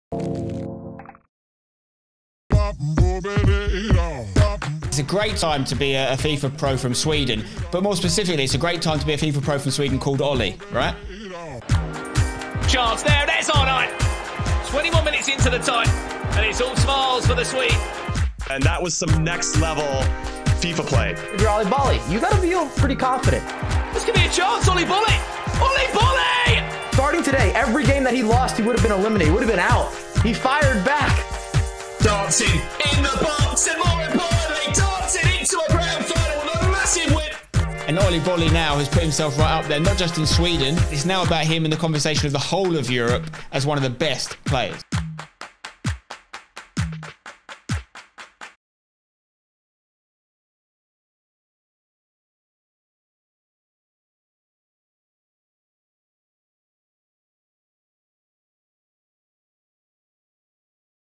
OliBoli - FIFA Global Series 21
Oliver "OliBoli" Uttgren is a Swedish FIFA Pro who played for Mkers in FGS 21. OliBoli has been part of the Swedish FIFA scene and started to make noise in FIFA 20, winning several Futhead TNF competitions. His hard work finally paid off on the largest stage, winning Europe Qualifier 1 on PS4 in FGS 21 and taking home the trophy at the FIFAe Club World Cup 2021 for Mkers alongside teammate Prinsipe. He capped off the FGS 21 season by placing top 8 in the Europe PS4 Playoffs and qualifying for the FIFAe World Cup before it was cancelled. p